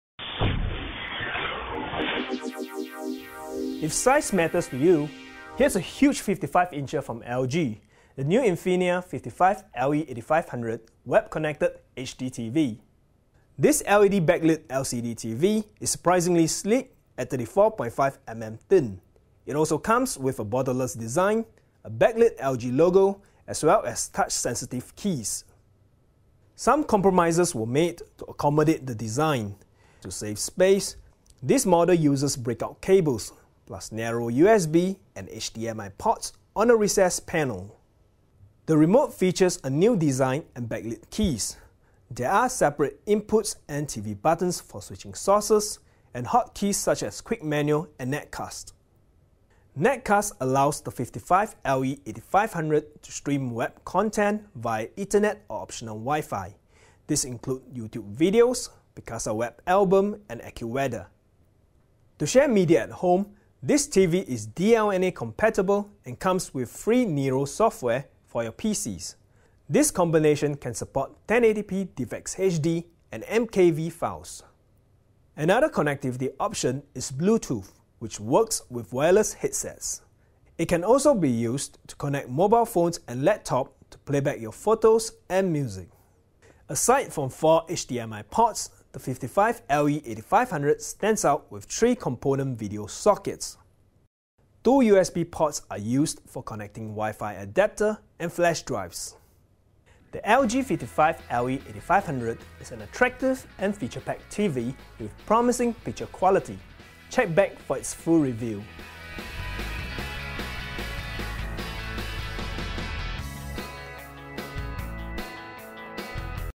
CNET ASIA REVIEW -- LG INFINIA 55" LE8500
CNET ASIA TAKES A FIRST LOOK AT THE INFINIA LE8500 -- AND GIVES IT THE THUMBS UP.